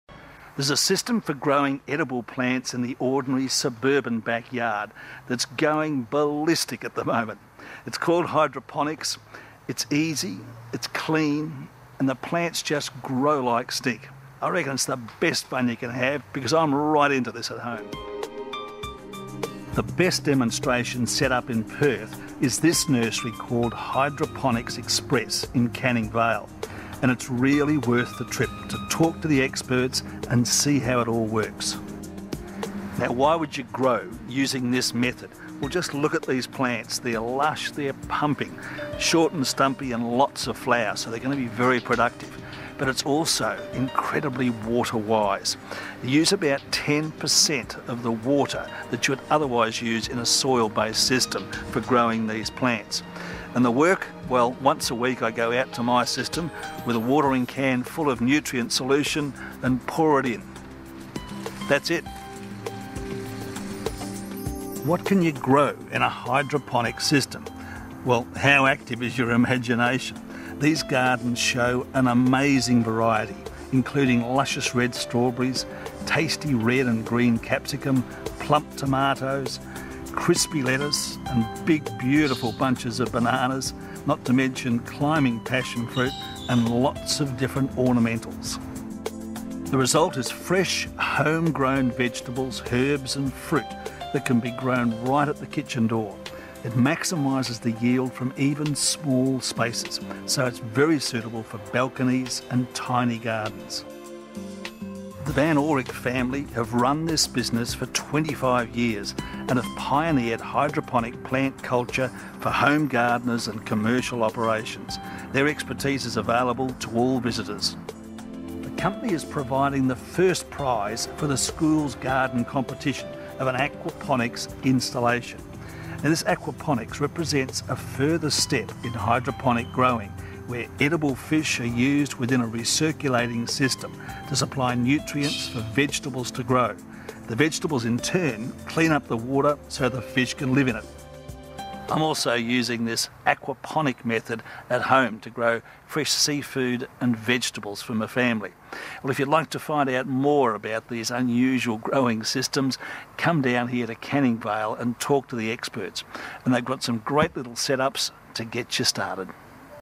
Hydroponic Perth WA - Hydroponic Display Centre Canning Vale
Neville Passmore of the Garden Guru's takes a tour of Perth's Hydroponic Xpress and Aquaponics WA massive 2000 plus sqm showroom and display centre in Canning Vale, Perth, Western Australia. Open 7 days. Bring the family and you'll be amazed.

Neville explains how to set up your own hydroponic or aquaponics system at home. Hydroponics offers superior quality and production while Aquaponics is the combination of hydroponics and aquaculture. Open 7 days. Hydroponic Xpress and Aquaponics WA invite you to visit with us to discuss your requirements.